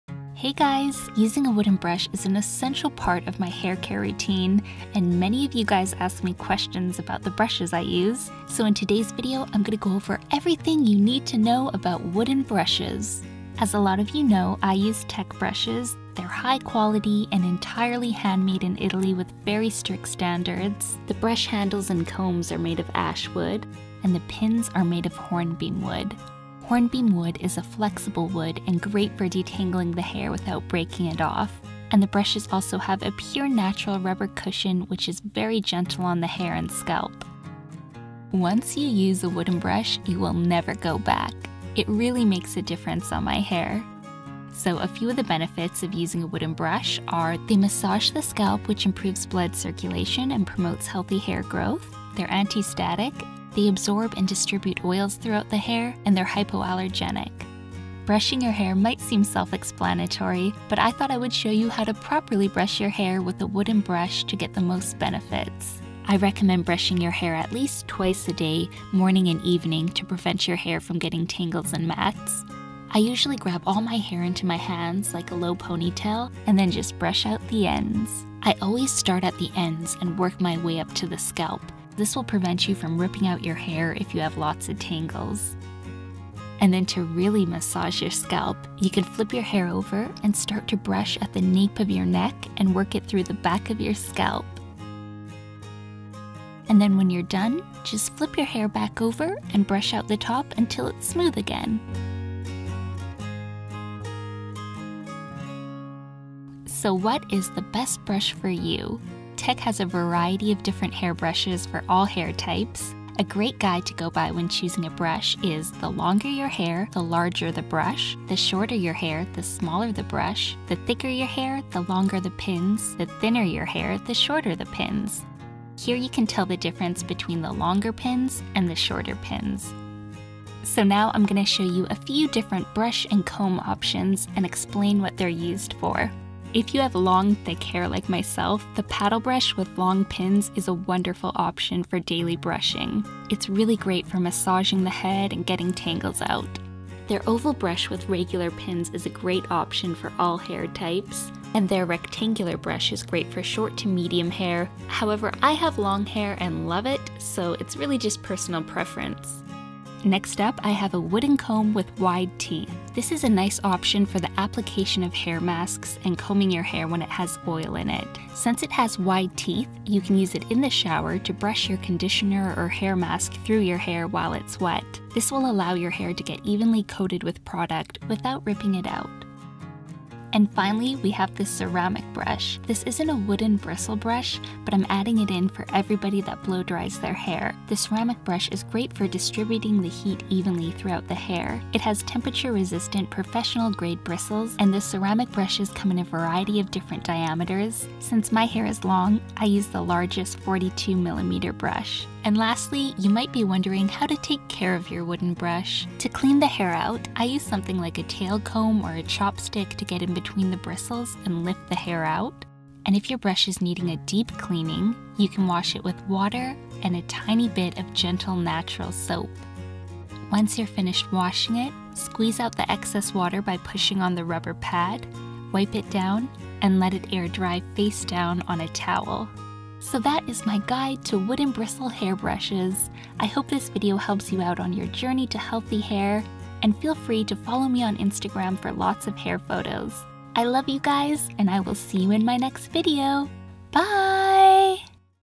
Tek | Haircare Routine: Choosing a Brush, Uses, Benefits and Care
How to use a wooden Tek brush by Sarah Nagel (@holistichabits). All our vegan and plastic-free brushes are designed to respect nature and your hair health. This is how to use them and take care of yourself.